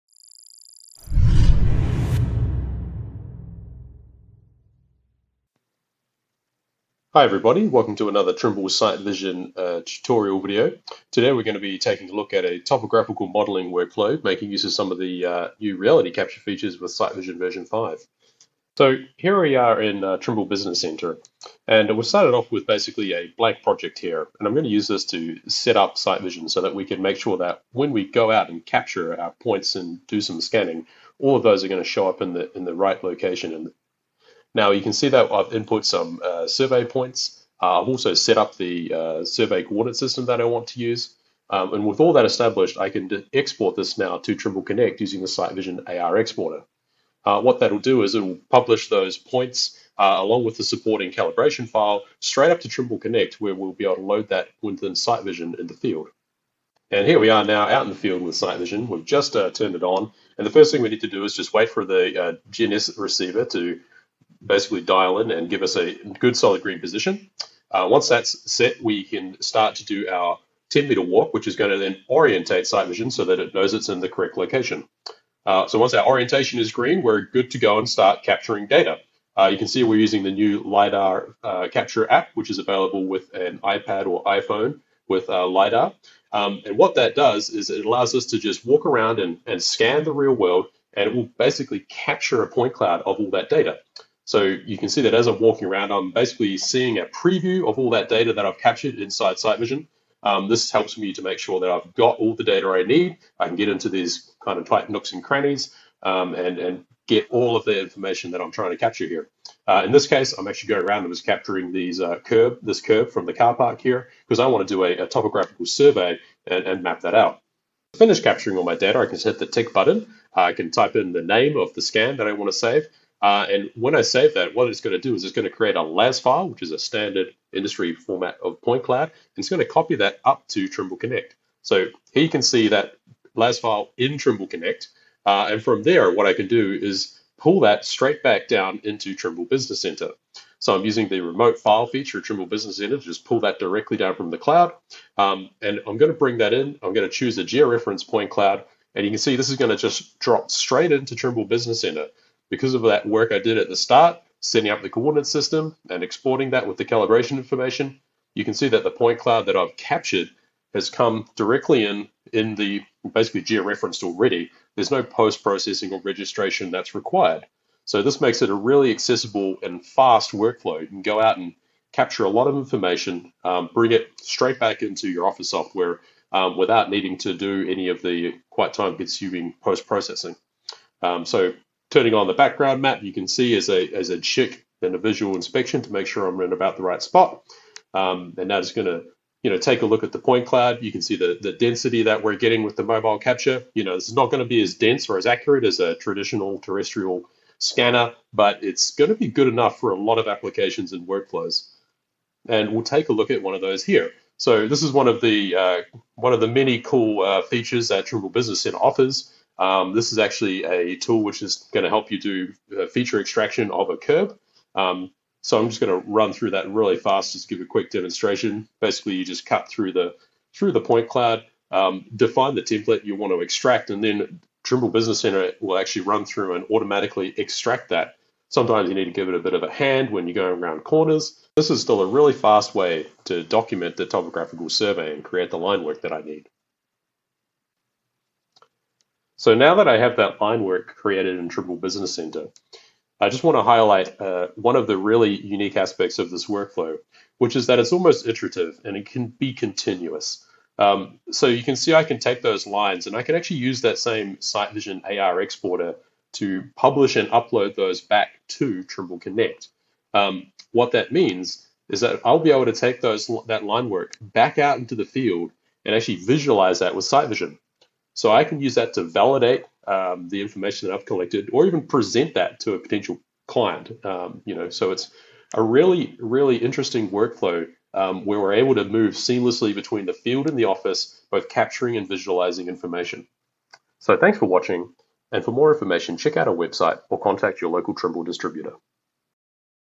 Topo Modelling with SiteVision Reality Capture
Leveraging the powerful LiDAR reality capture capability, see how SiteVision can quickly capture georeferenced data in the field.   Then quickly turn that into deliverable data using the incredible point cloud processing tools available in Trimble Business Center.

00:00 Setup
00:55 3D scanning in the field
02:07 Scan data review in the office
03:21 Feature extraction from scan data
04:30 Visualize the created data in the field